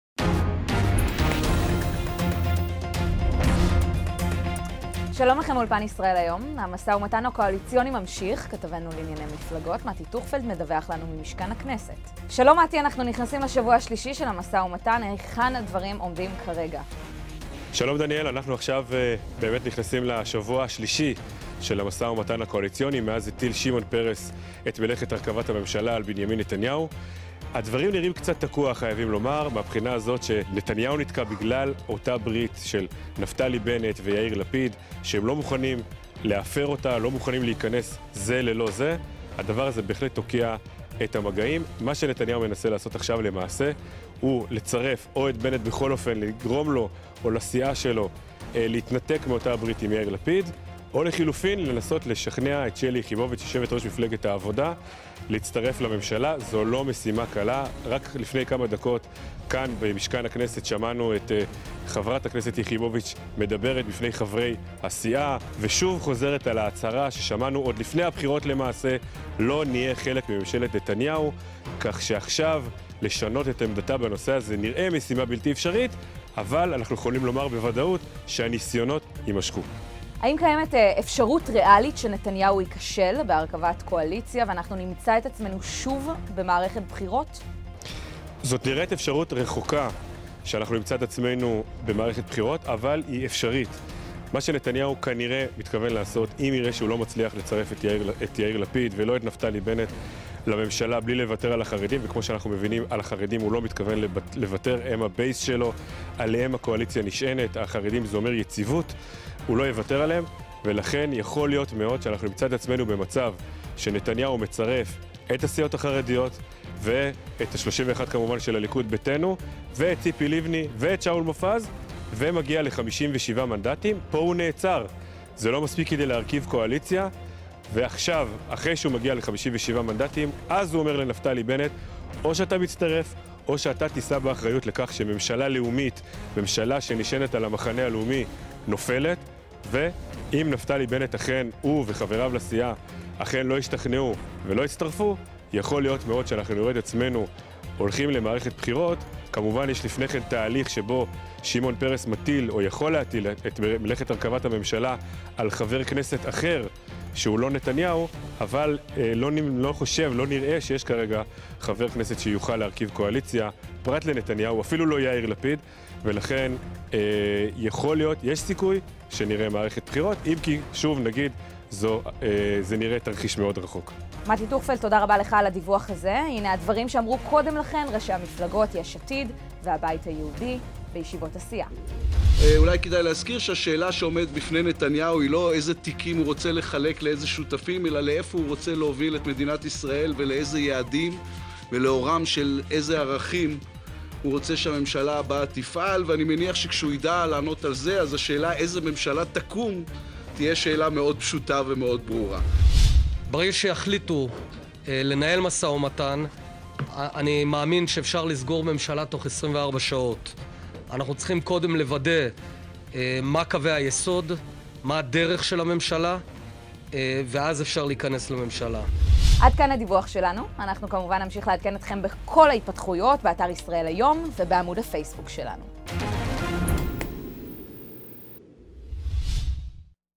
ישראל היום | הדיווח של מתי טוכפלד על המו"מ הקואליציוני
למרות הדיווחים על קיפאון במו"מ הקואליציוני, יו"ר הבית היהודי ממשיך להצהיר על רצונו להצטרף לממשלה • "חשוב לנו לדבר על עקרונות לפני חלוקת תיקים", אמר בנט • לפיד: "השאלה שעומדת בפני נתניהו היא לאן הוא רוצה להוביל את המדינה"

מגישה: דניאל רוט
צילום: זוהר קליגמן וערוץ הכנסת